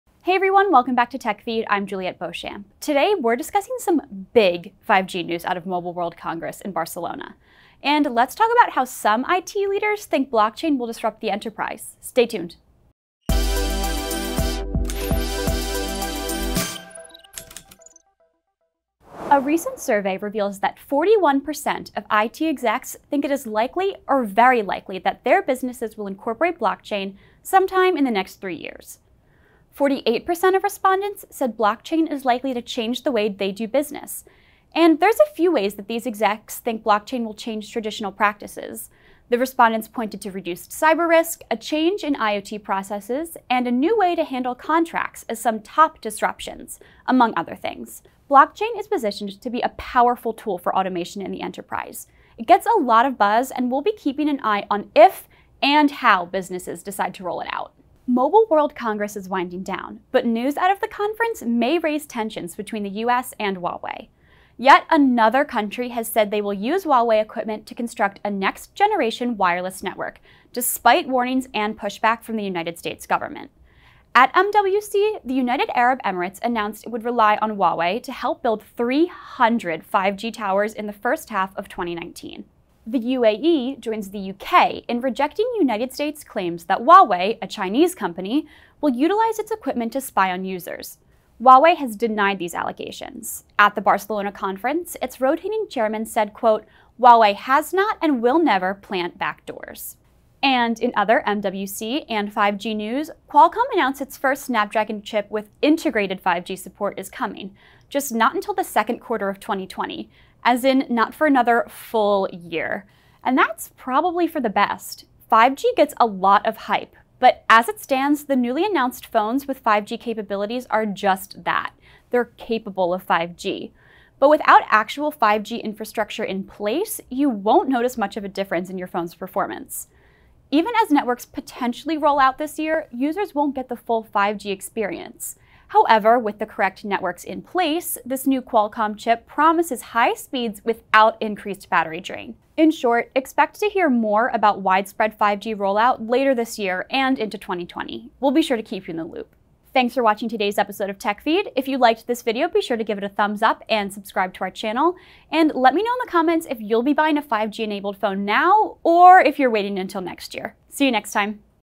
5G networks in 2019? Plus, blockchain gains interest | TECH(feed)
New 5G phones have been unveiled at Mobile World Congress in Barcelona, but will users even be able to access 5G networks? And another country rejects U.S. allegations that Huawei spies on users. All that and more on today's episode of TECH(feed).